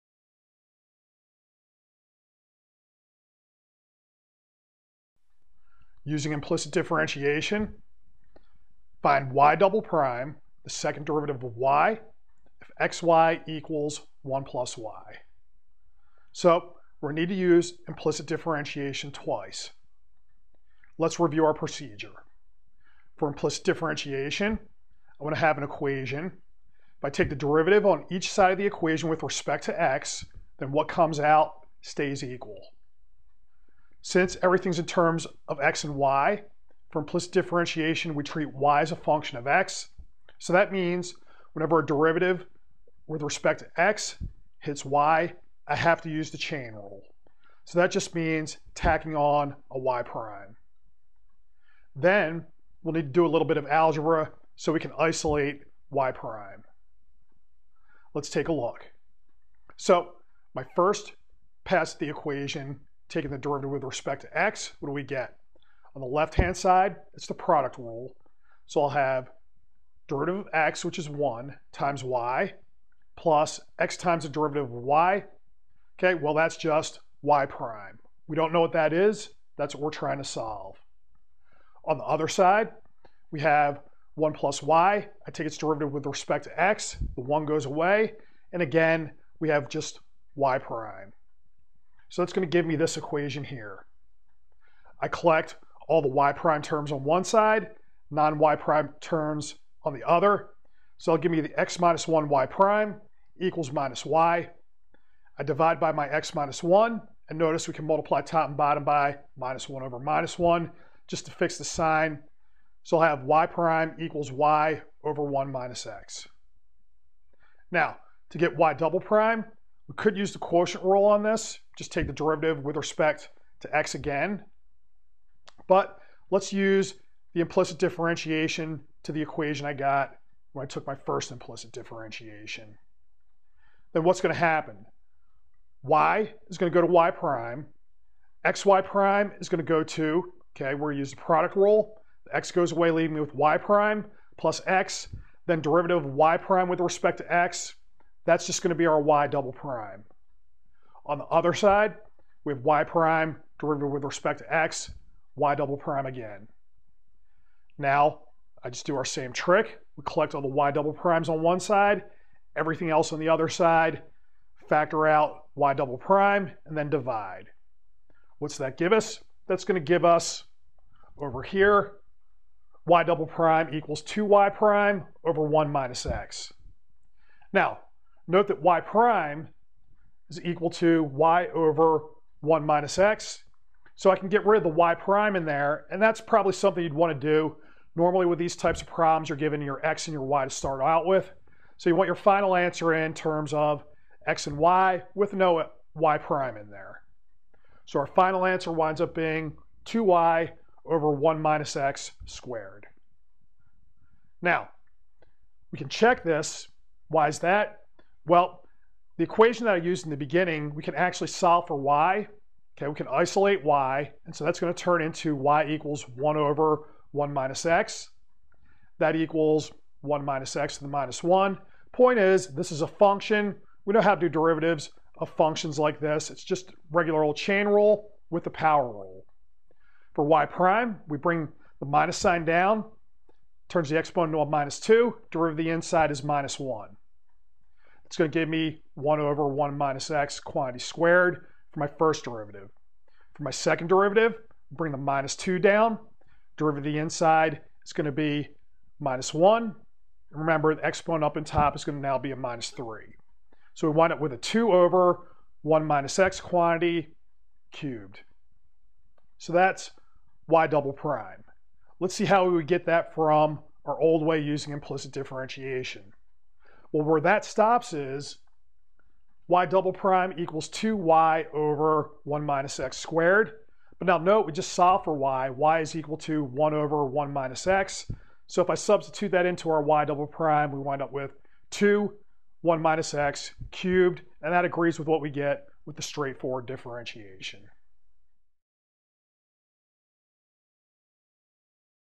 Implicit Differentiation 5 - Higher Derivatives
Calculus:   Find the second derivative y"  for the equation  xy = 1 + y  using implicit differentiation.